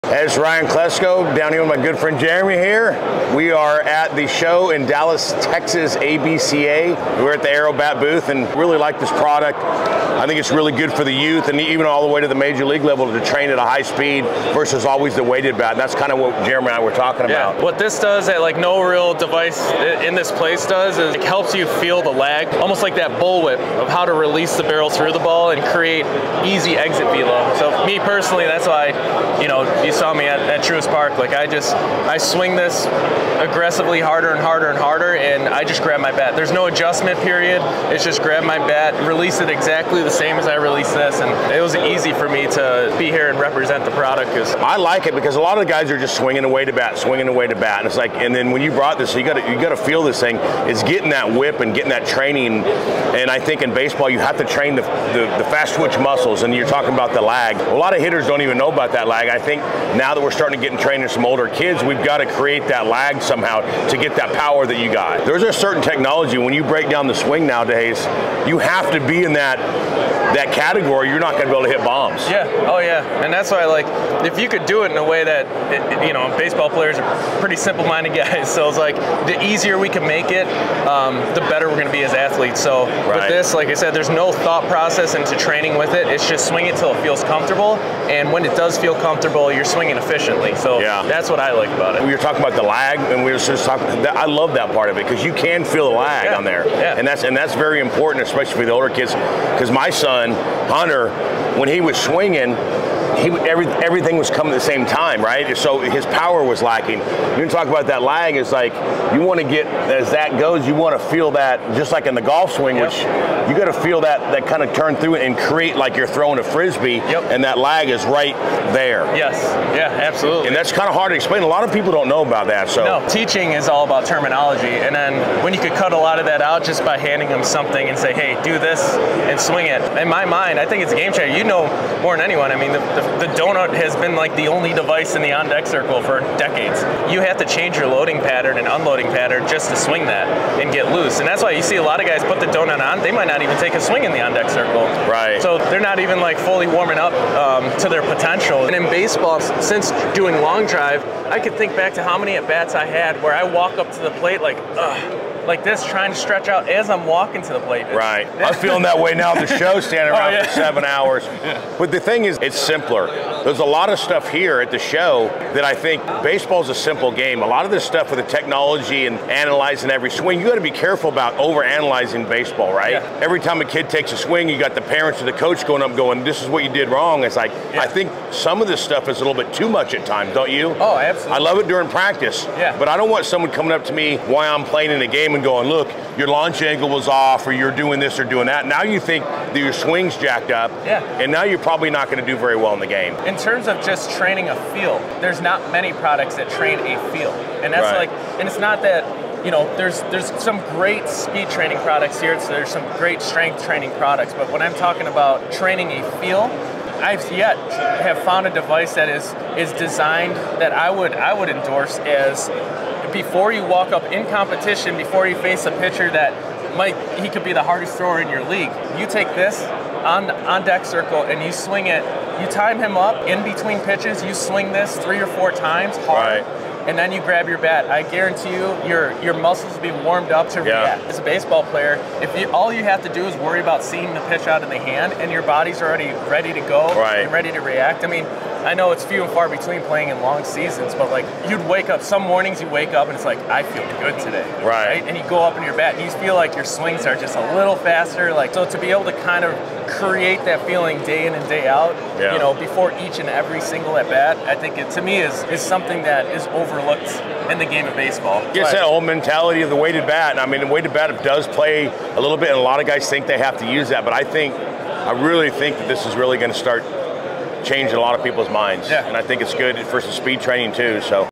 Aero-Bat - The Ryan Klesko and Jeremy Nowak endorsement!
The Aero-Bat is the best warm up and swing speed device ever invented for baseball.   Warm up quicker and use that saved energy at the plate.